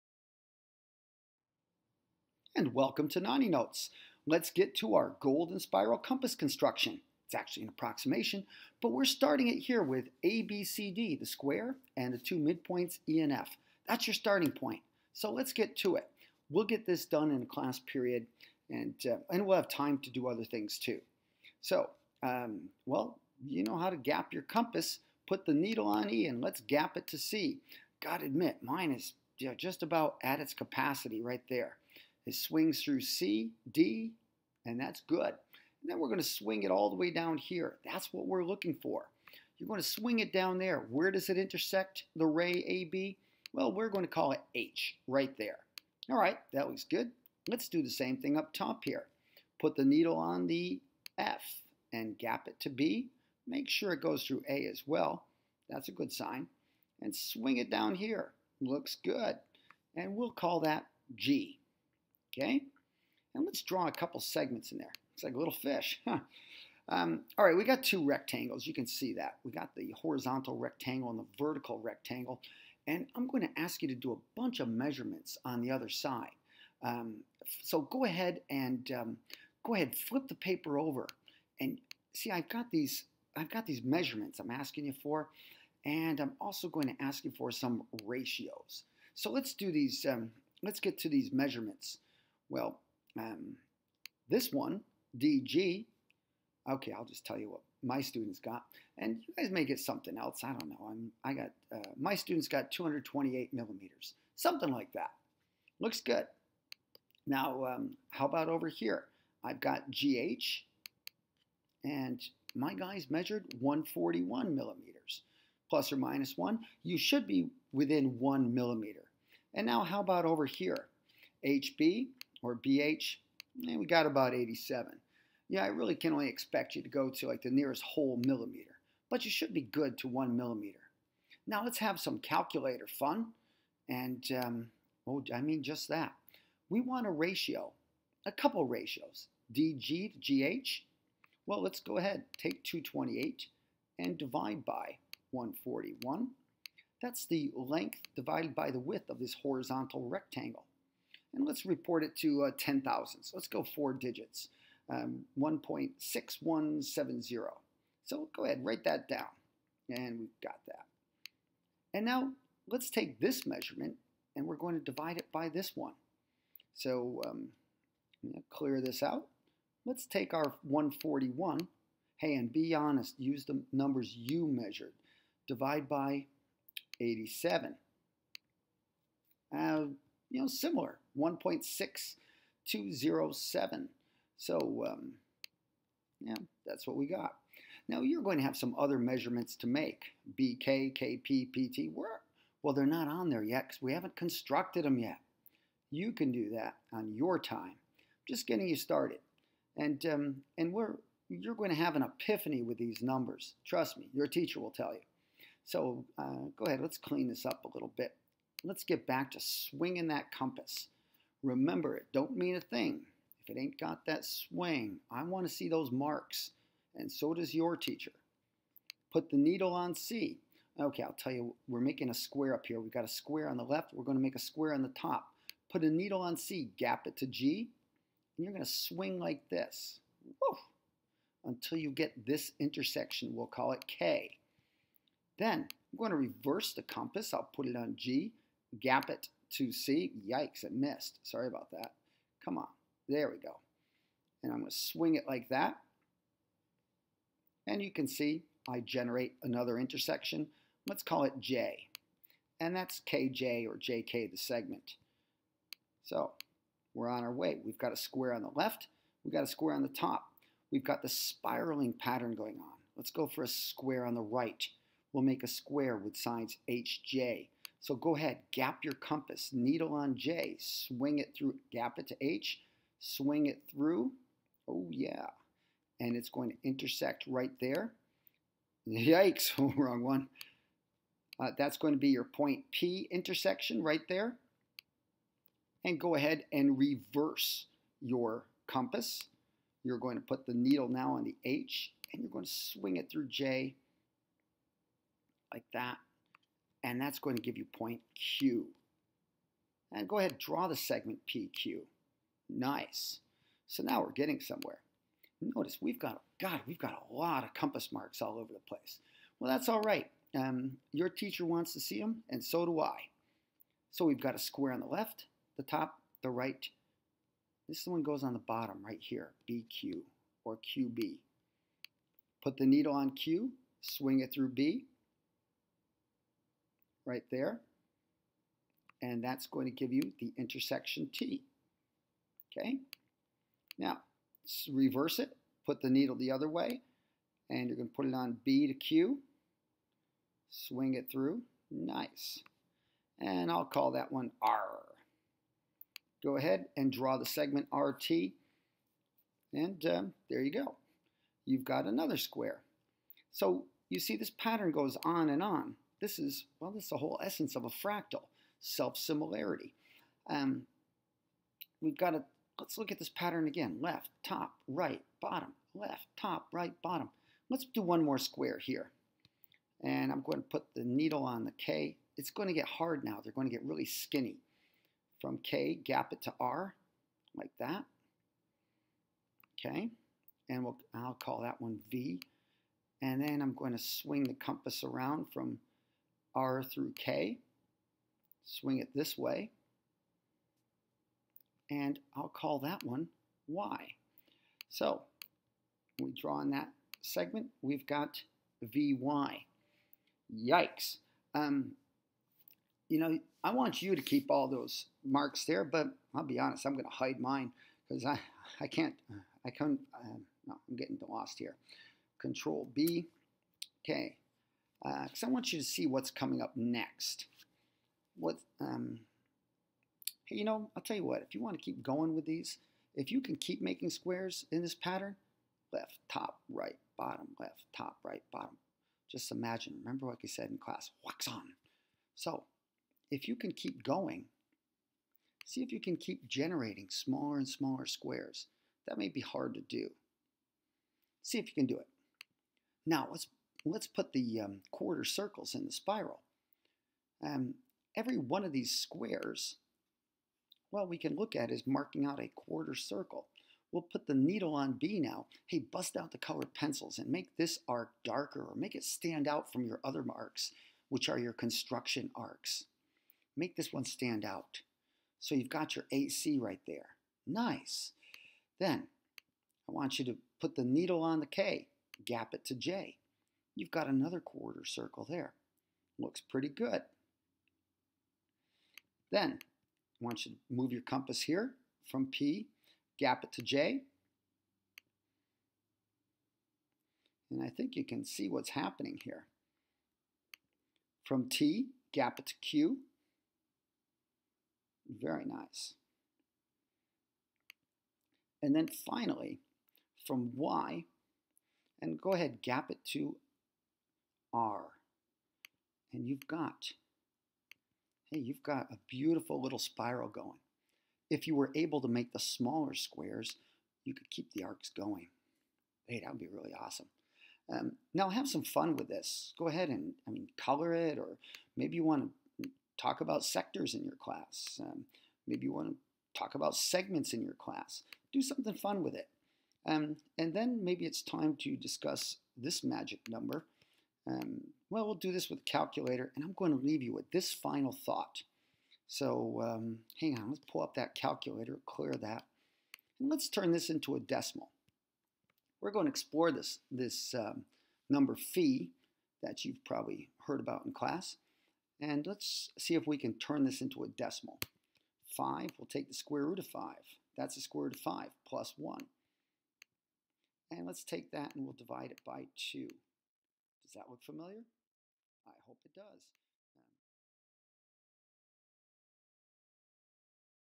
Golden Spiral Compass approximation
This compass construction requires less than 30 minutes.
We perform this activity in chapter 6 (Similarity) of McDougall Litell textbook

We explore the concepts of ratios, similarity, and fractals.